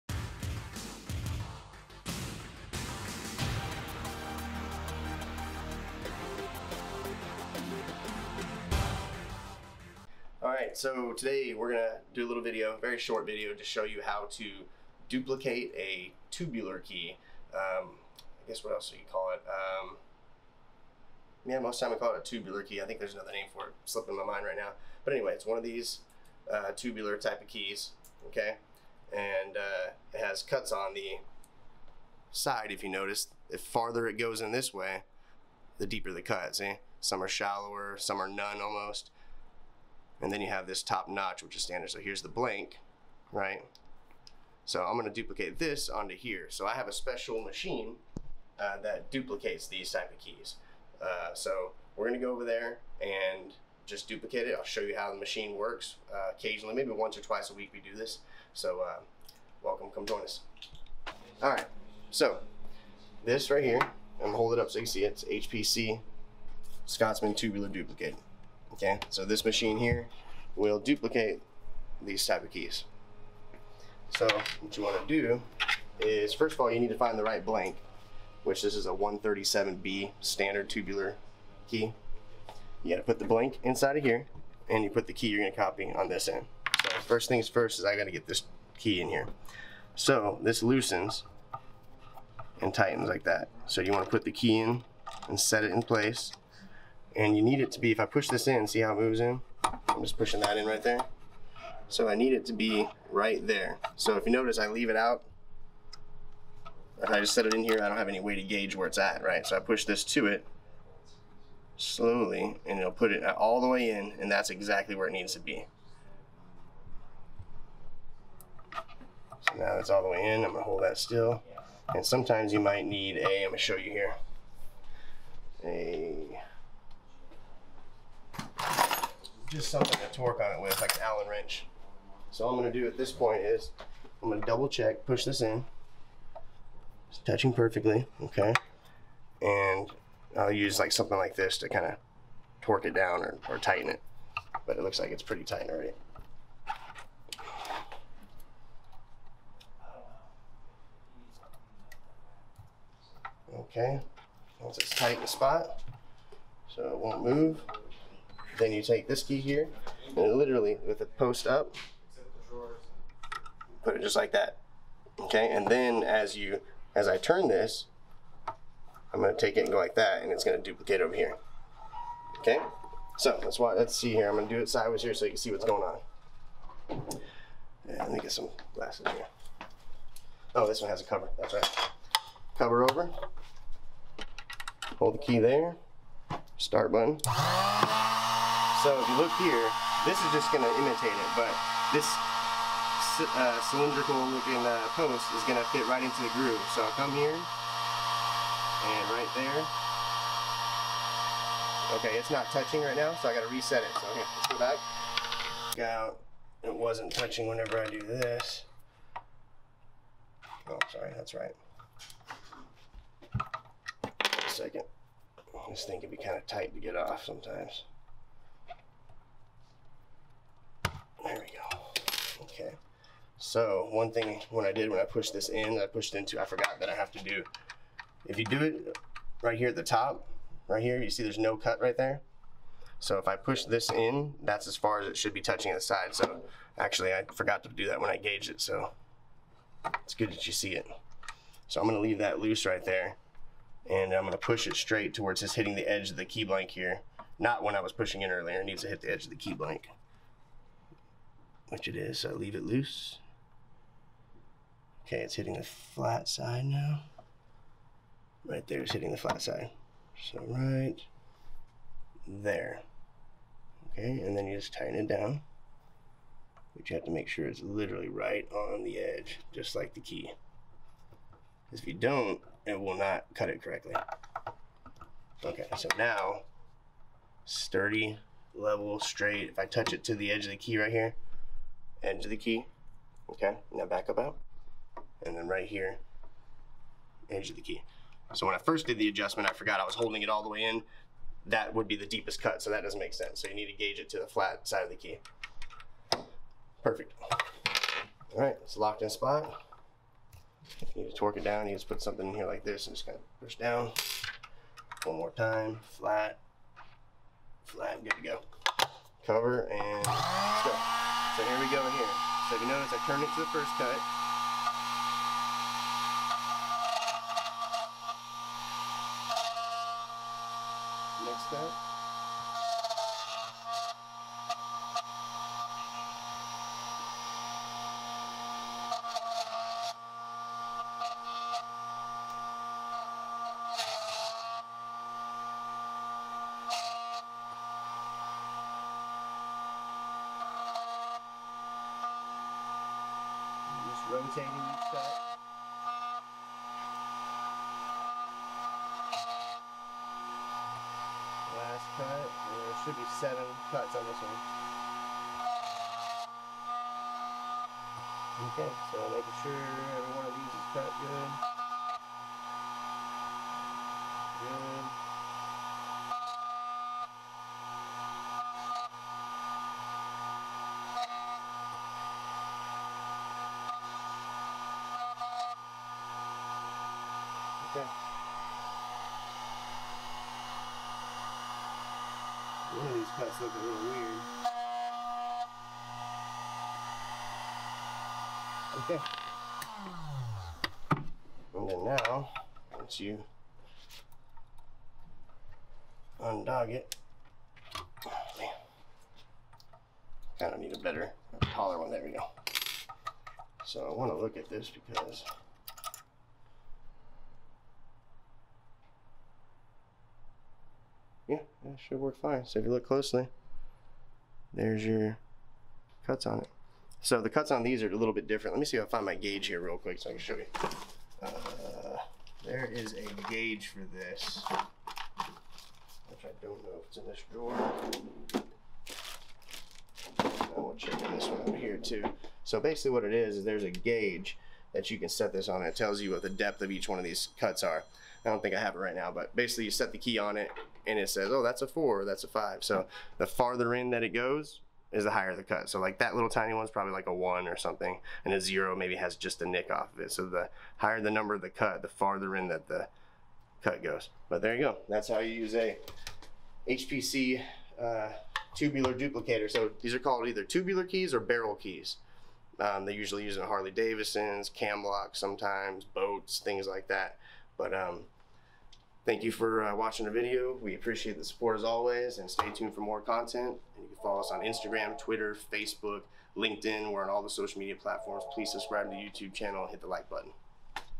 Need A New Barrel Key? Watch How To Duplicate A Tubular Key
A Tubular key can go by a few different names. Barrel keys, radial keys, circle pin tumbler locks, and the trademarked "Ace lock" all refer to a system in which the pins are aligned to face the operator in a circle instead of sitting vertically in a line over the key.

Copying a tubular key is a notorious task for those outside the industry, with many people believing they can't have copies of the key made when turned away at big-box stores; but again, your local locksmith can save the day.

Uriah goes over the HPC Scotsman tubular key duplicator and how to operate the device to copy a customer's tubular key. This device is fairly standard to the industry, and should be a process repeatable by your local locksmith!

How can I be sure that I'm hiring a legitimate local locksmith?

What other forms of security are out there to help protect my business?